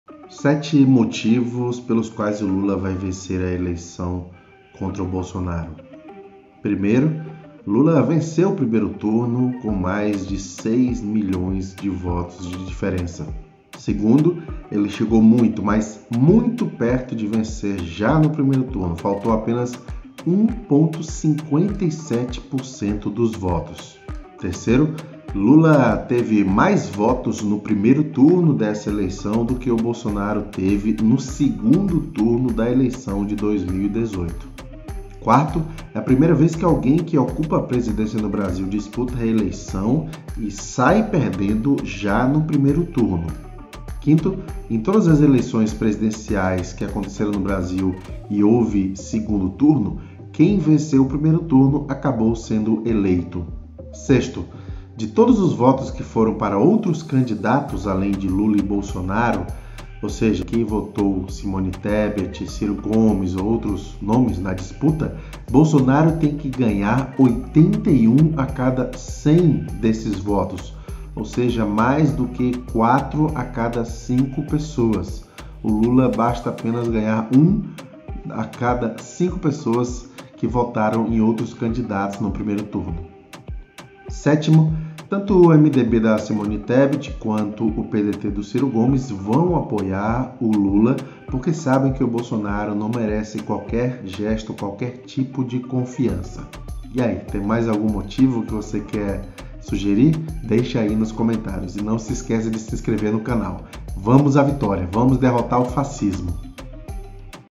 Sete motivos que apontam a vitória do Lula contra Bolsonaro no 2° turno
03/10/2022 - Listo sete fatos que apontam a vitória de Lula contra Bolsonaro no 2° turno das eleições presidenciais do dia 30 de outubro.